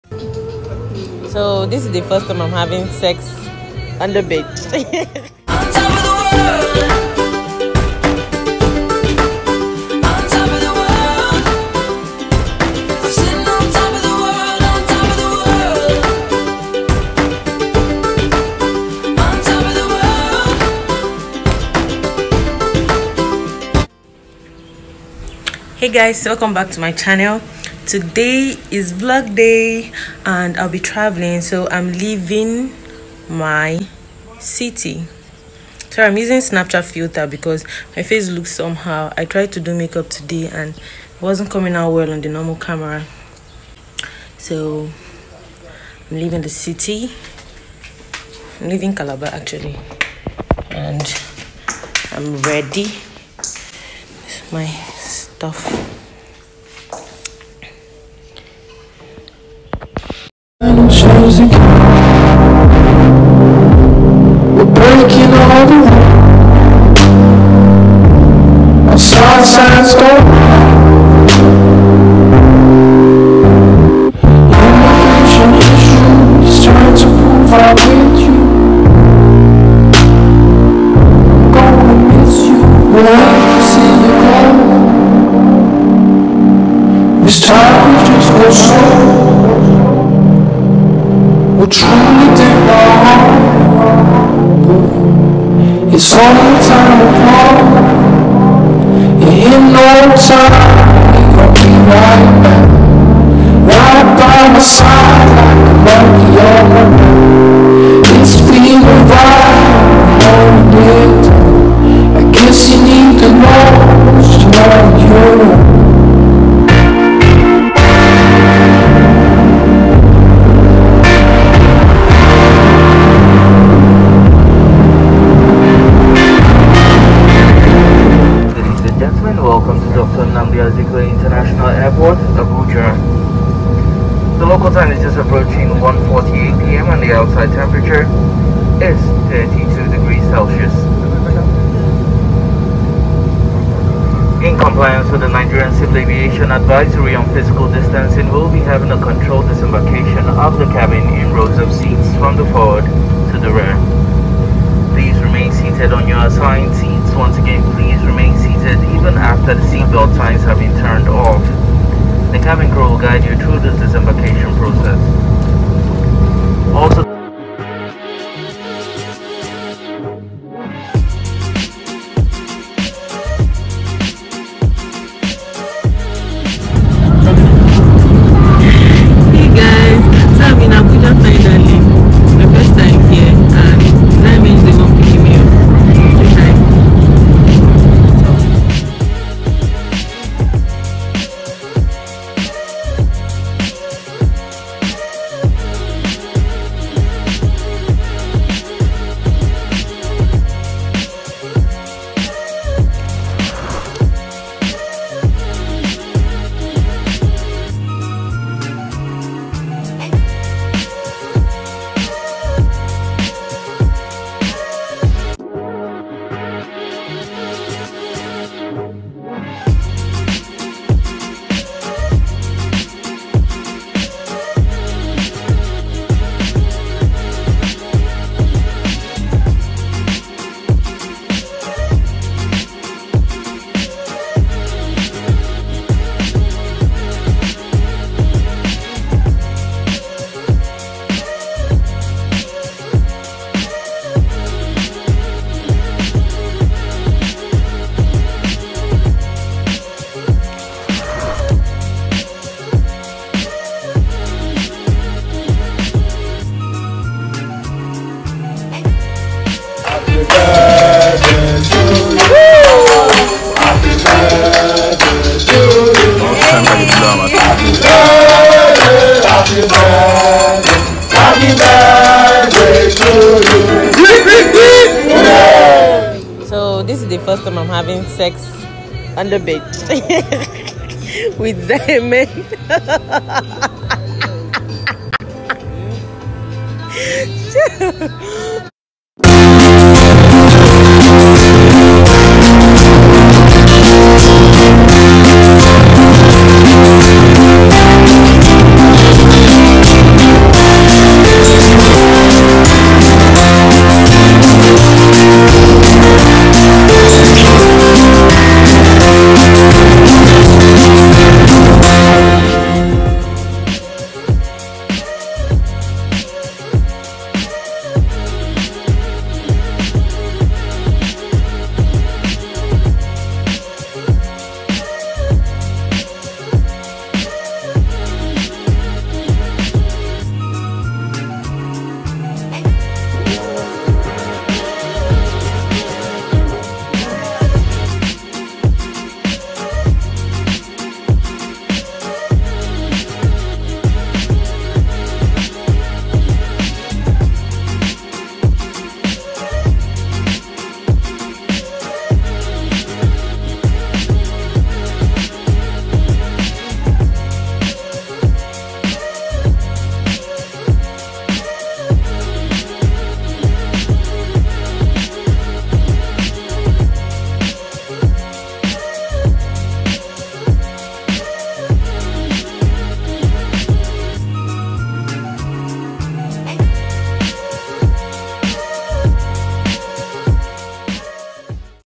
Relocating to Abuja/Emotional/Depression/best friend’s birthday
Hello, it’s been over a month I posted because I was passing through a whole lot, and relocating was one of the self healing step I took for myself... it felt strange going to a city I know absolutely nobody
Zahemen traveled back to the states soon after😢
I’d be posting more vlogs
Abuja is a pretty cool city.